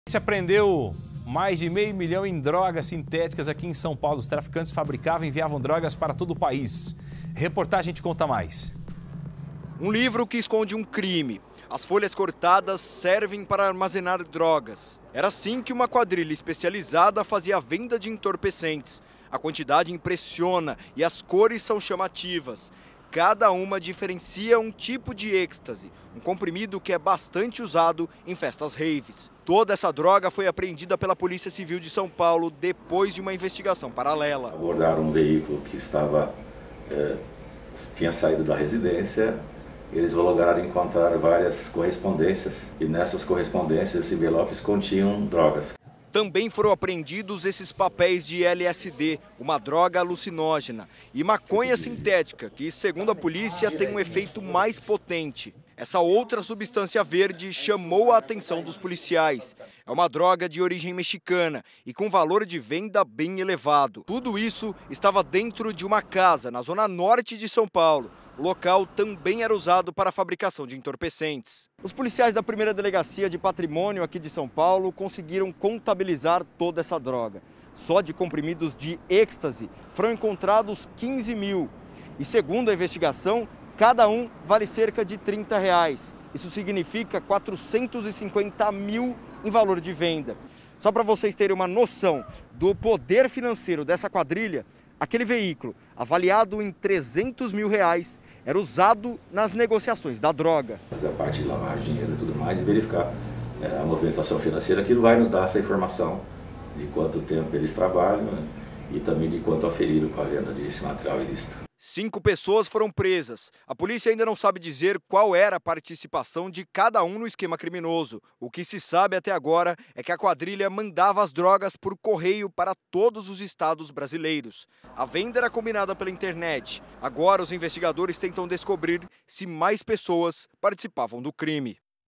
Polícia apreende R$ 500 mil em drogas sintéticas em São Paulo
Após investigação, a Polícia Civil de São Paulo apreendeu cerca de R$ 500 mil em drogas sintéticas em uma casa na zona norte da capital. A quadrilha enviava as drogas por correio para todos os estados brasileiros. Cinco pessoas foram presas na ação.